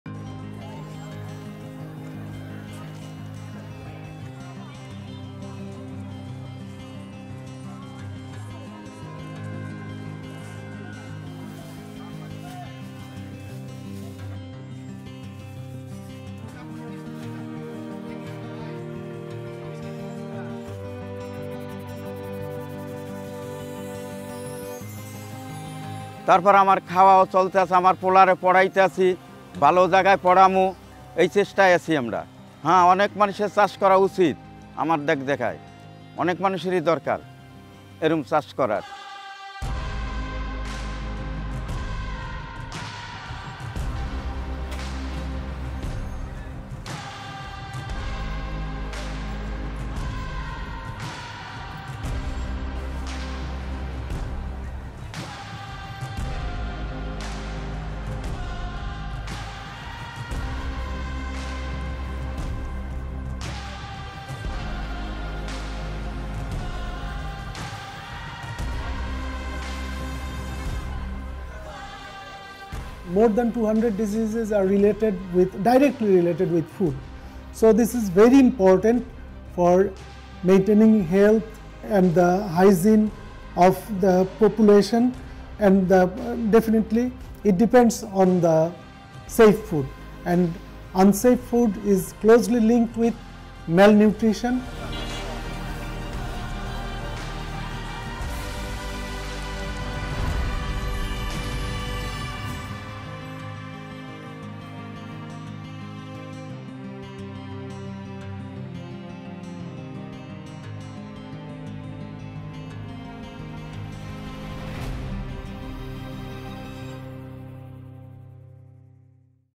كيف يمكننا أن نضمن سلامة الأغذية في المدن السريعة النمو؟ (sq)
كيف يمكننا أن نضمن سلامة الأغذية في المدن السريعة النمو؟

في بنغلاديش، تقدم منظمة الأغذية والزراعة
الدعم التقني
للحكومة
لتعزيز
التغذية الأفضل
من خلال أغذية أكثر أمانًا.

والتغذية الأفضل حاسمة الأهمية
لتحقيق أهداف
التنمية المستدامة.

غير أن التوسّع الحضري السريع
لدكا يطرح
تحديات أمام
سلامة الأغذية
والتغذية.

والزراعة الحضرية تجعل
الأغذية الآمنة والمغذية
أقرب إلى منازل العديد
من سكان المدن.

السيد Mohammad Awwal
بُستاني في المناطق الحضرية، مواكالي
بات بإمكاننا الآن أن نحصل على طعام جيد. وأستطيع
تحمل تكاليف تعليم ابني،
ونأمل أن يحصل على
درجات جيدة.
وينبغي لسكان المدن الآخرين
أن يبدأوا في ممارسة الزراعة
بعد مشاهدة ما أفعله.
فعلى العديد من الناس أن يشرعوا في عملية الغرس.
بالنسبة إلى الأسر المعيشية الأفقر،
تظل أسواق المنتجات الطازجة في دكا
أيضًا ضرورية.
ويشتري 95 في المائة من الفقراء في المناطق
 الحضرية أغذيتهم
في هذه الأسواق.
وتتمثل إحدى الأولويات في تحويل
أسواق المنتجات الطازجة التي كانت
مكتظة وغير صحية.
وتعمل منظمة الأغذية والزراعة مع
الشركاء لتحويل
أسواق المنتجات الطازجة هذه
لكي تقدم
أغذية آمنة وطازجة بقدر أكبر.
ويساعد تحسين الصرف
وإدارة النفايات
وتجهيز الأغذية
جميعها على الحد من
مخاطر الأمراض المنقولة عن طريق الأغذية.

وهذا يتيح فرصًا أفضل
للبائعين والمورّدين
والمستهلكين.

العميد الدكتور Zobaidur Rahman
كبير مسؤولي الصحة في
المجلس البلدي لمدينة دكا الشمالية
ترتبط أكثر من 200 حالة مرض
ارتباطًا مباشرًا بالأغذية.
لذا، فإن هذا أمرٌ مهمٌ للغاية
للحفاظ على الصحة
والنظافة الشخصية
للسكان.
وهذا يعتمد حتمًا
على توفير الأغذية الآمنة.
والأغذية غير الآمنة ترتبط ارتباطًا وثيقًا
بسوء التغذية.
إن تحقيق تغذية أفضل للجميع
يساعد على الحد من الفقر

والجوع وانعدام المساواة.

In the Asia-Pacific region, FAO is working with its Member Nations to help them advance to the 2030 Agenda by achieving Four Betters: better production, better nutrition, better environment and a better life – leaving no one behind. This short video looks at how urban farming and transforming once unhygienic urban markets in Bangladesh’s capital Dhaka is providing access to safer and nutritious food.